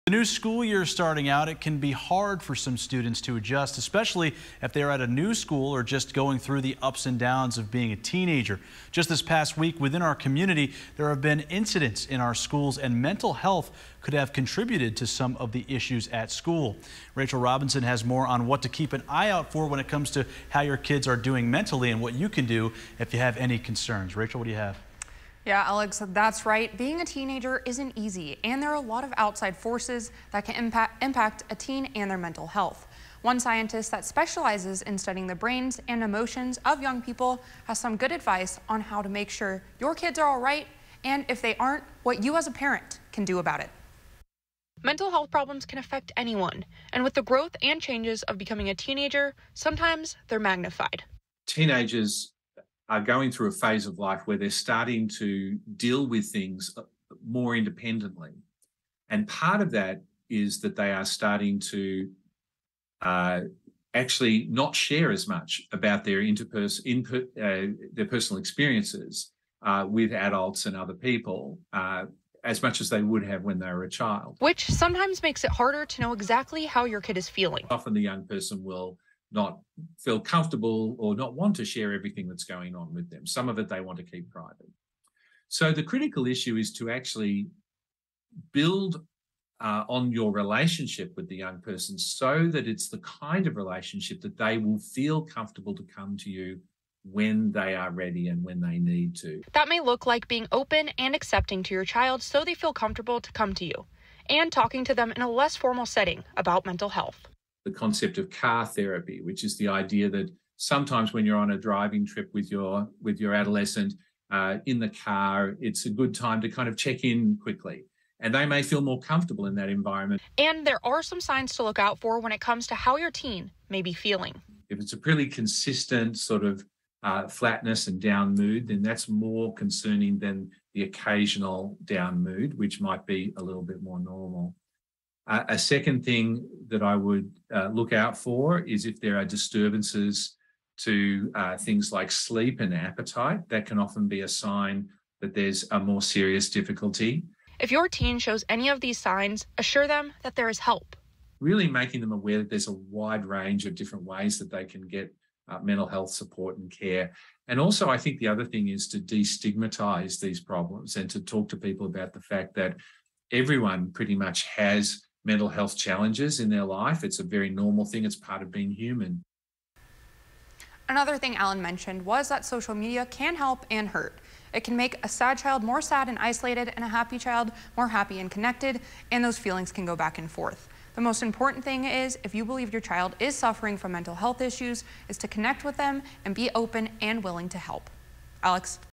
Supporting teenagers and their mental health during the school year
There are signs to watch out for to make sure your teen is okay.